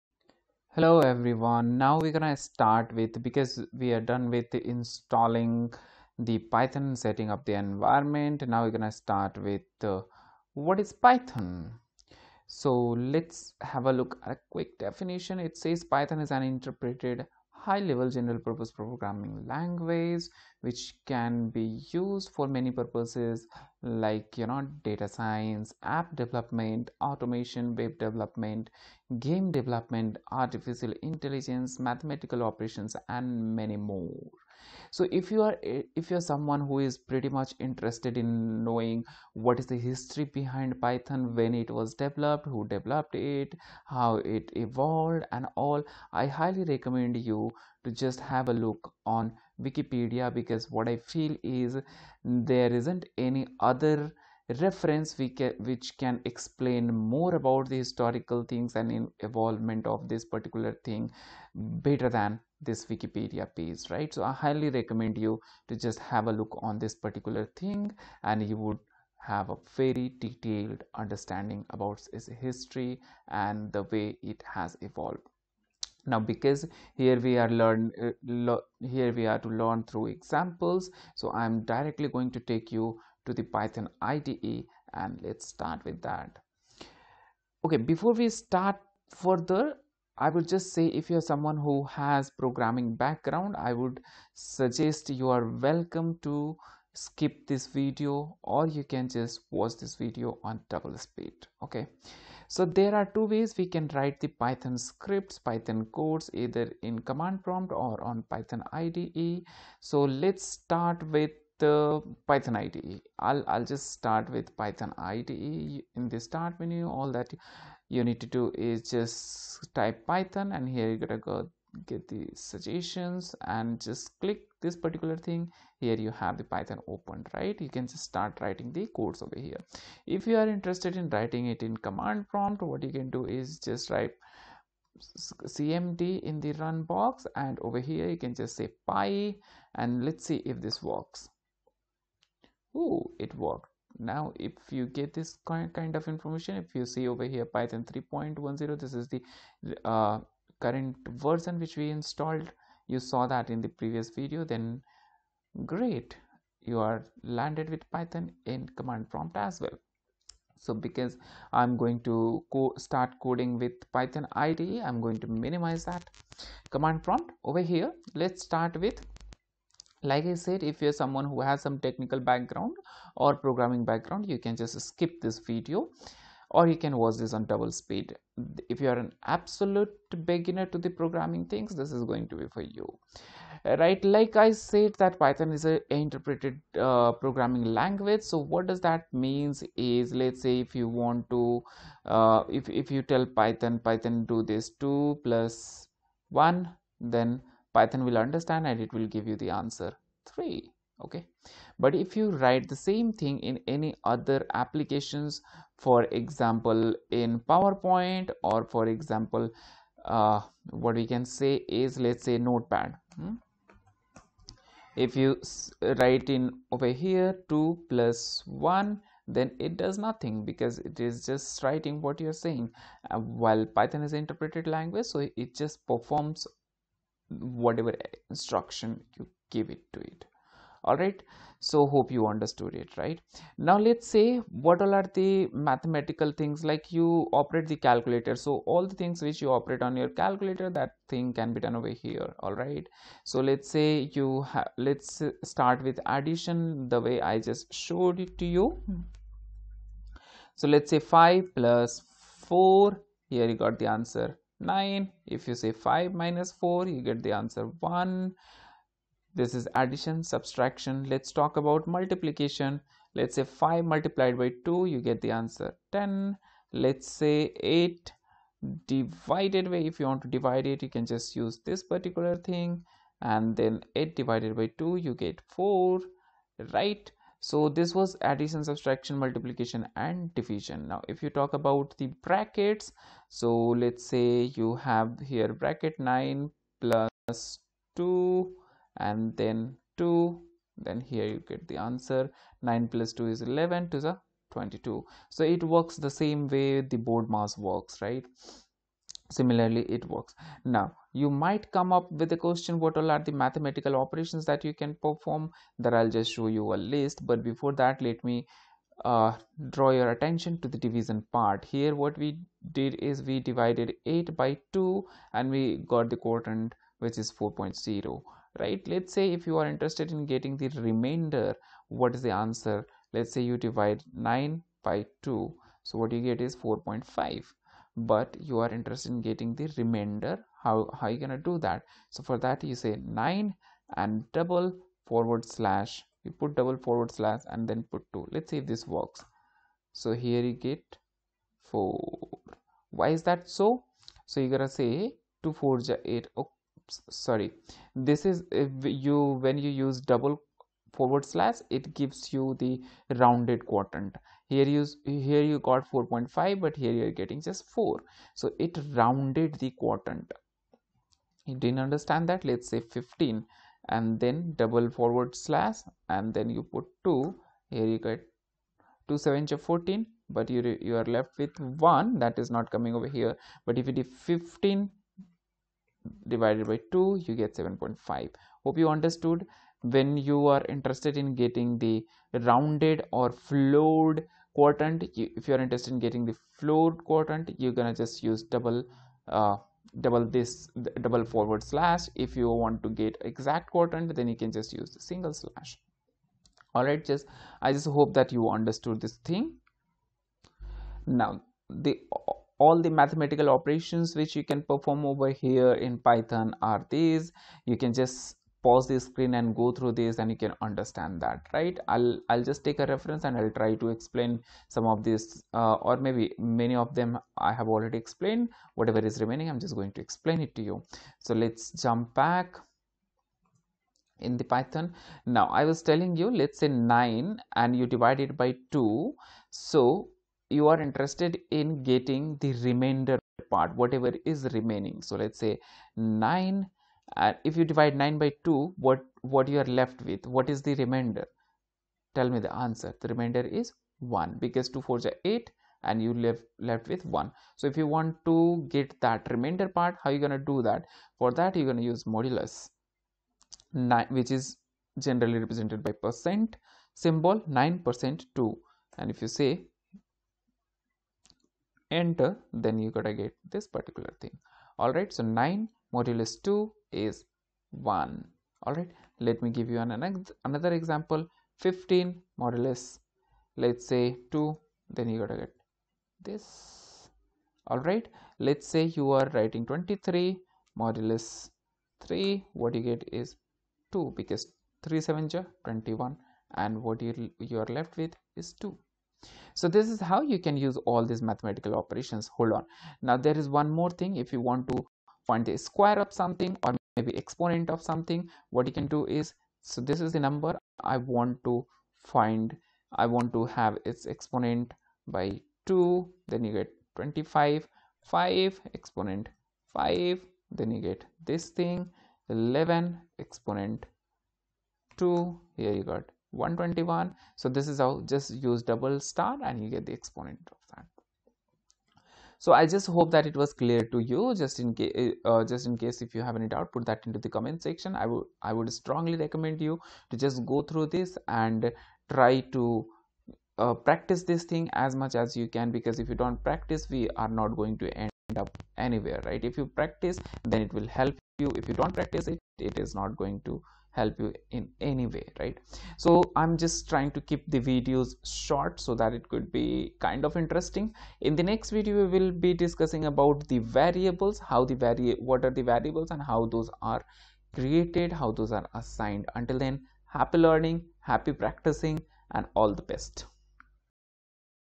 Python Tutorial - 2|Introduction to Python | Python for Beginners | Learnerea
Python Tutorial - 2|Introduction to Python | Basic Mathematica l Operations| Learnerea

00:00 - Introduction
00:12 - What is Python and what is it used for
00:51 - What does Wikipedia says about Python
01:55 - Starting with Python IDE
02:14 - Starting Python in Command Prompt
03:05 - What does an interpreter means
04:12 - Performing basic mathematical operations in Python
05:41 - List of mathematical operations/operators performed in Python